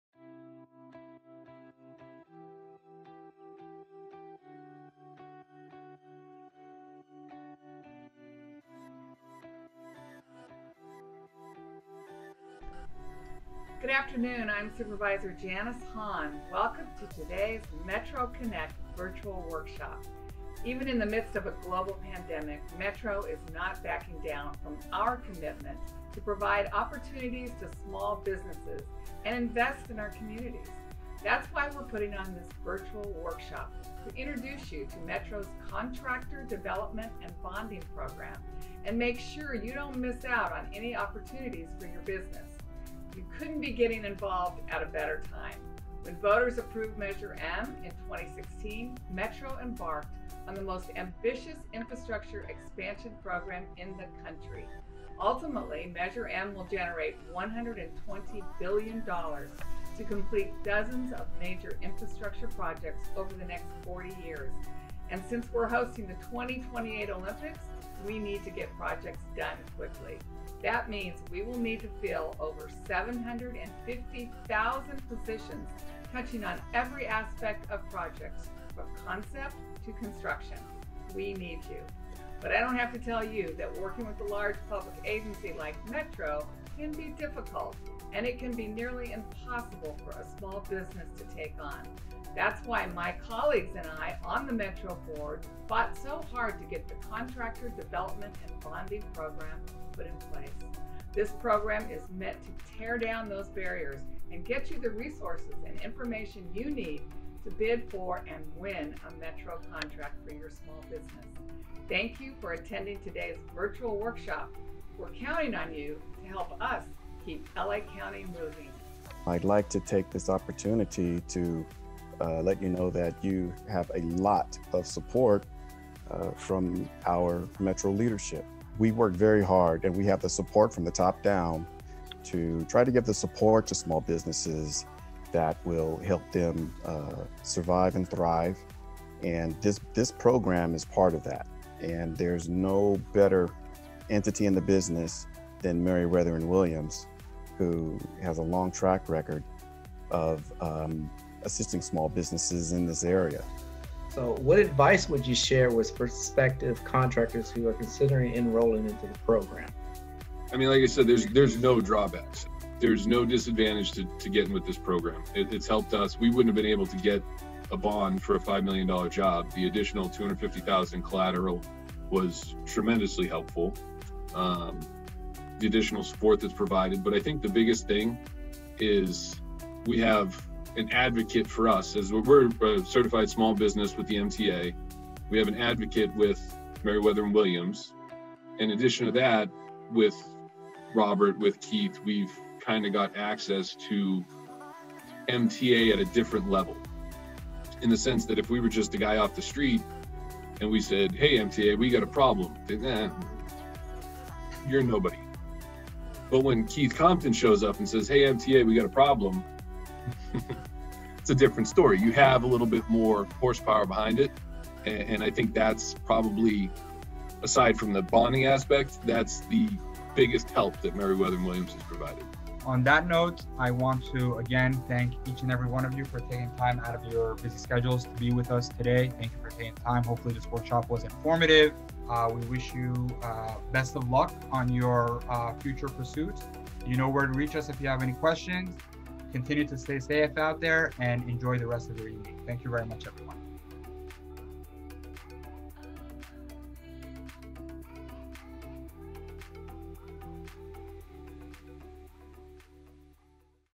Navigating Metro Contracting Opportunities
Metro's Contractor Development and Bonding Program (CDBP} is designed to assist contractors with their bonding capacity and business growth. In this webinar, participants heard first-hand from CDBP and Metro about the program and existing construction project opportunities.

This was a free webinar for contractors who are:
• Interested in doing business with Metro
• Would like to be SBE, MBE, DVBE, DBE, LBE certified
• Looking to grow or obtain bonding
• Interested to learn about upcoming Metro construction opportunities

Become part of the Metro Contractor Development and Bonding Program (CDBP} to receive business assessments, bonding assistance, technical support, training through workshops, and Prime Partner networking opportunities.

Janice Hahn, Los Angeles County Supervisor

LA Metro - Keith Compton, Director, Capacity Building & Integration, Diversity & Economic Opportunity Department

LA Metro - Miriam Scott, Director, Strategic Outreach Services, Diversity & Economic Opportunity Department

Metro and the City of Los Angeles Contractor Development and Bonding Program - Navid Barkhordar, VP, Merriwether & Williams Insurance Services

Metro and the City of Los Angeles Contractor Development and Bonding Program - Robert Lowery, Field Support Project Manager, Merriwether & Williams Insurance Services

SJN Builders, Inc. - James Bellofatto

For more information about CDBP’s services and to inquire about our next Metro workshop, please or contact our office at .